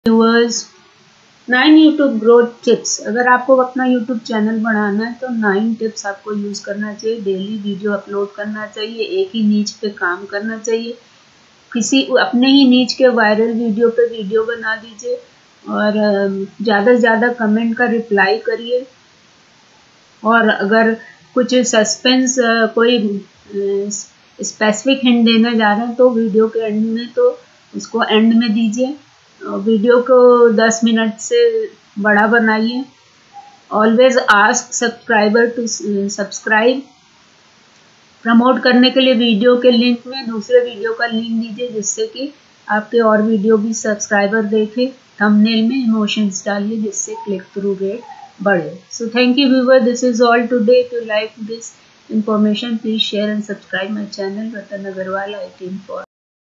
9 YouTube Growth Tips | RATAN AGARWAL IT INFORMER
9 YouTube Growth Tips
Daily Uploads
Build Videos Around A Single Keyword/Topic
Make Similar Videos to Already Viral video
Reply to as Many Comments as Possible
Hint at what will be shown later in video to increase audience
Make videos longer than 10 minutes
Promote other videos you have to create view loops
Use human emotions in thumbnails for high click through rate
more to learn-
1. Build Videos Around a Single Keyword/Topic.
2. Be Searchable.
3. Link to Yourself.
5. Use Your End Screen.
6. Engage with Your Audience.
7. Get Branded.
8. Promote Your YouTube Videos on Other Social Channels.
9. Post Great Thumbnails
10. Leverage YouTube Cards
11. Push for Subscriptions
12. Upload More
13. Use Analytics